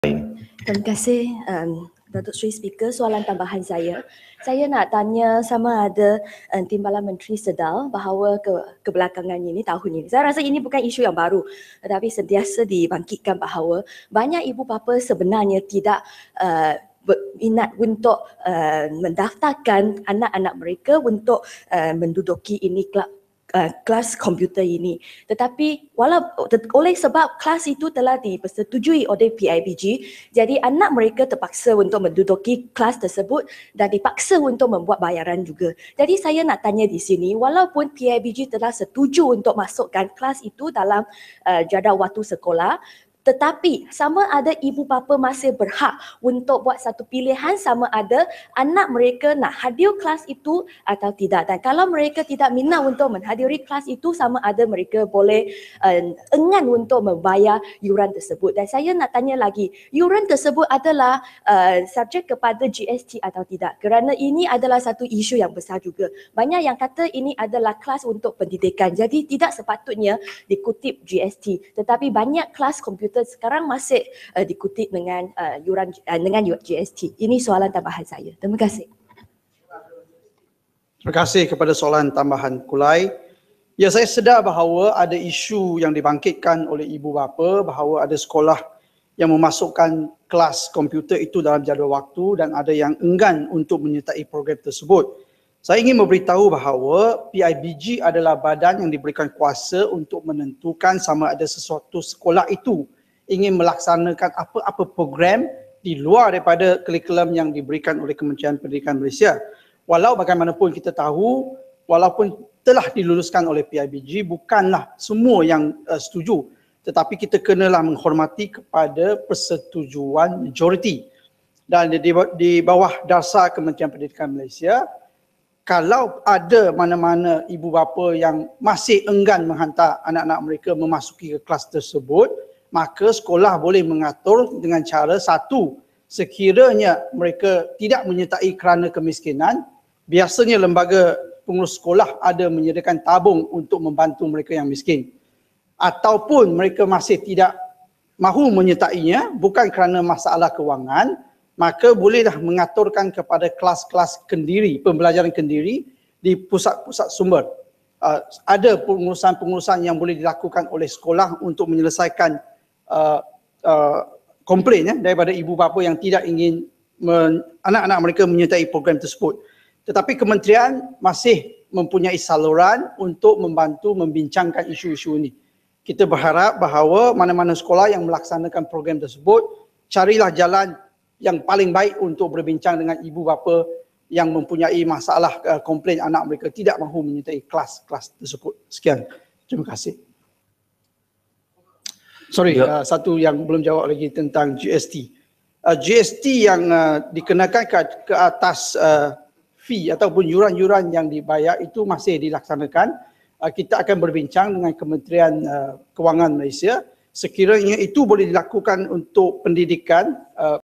(Kulai ) Teo Nie Ching | Pihak sekolah berhak memasukkan kelas komputer dalam silibus? [23.3.2016]
Teo Nie Ching  [ kulai] minta Menteri Pendidikan menyatakan sama ada pihak sekolah atau PIBG berhak untuk memasukkan kelas komputer dalam silibus dan mewajibkan ibu bapa untuk membayar fi tuisyen kelas komputer tersebut.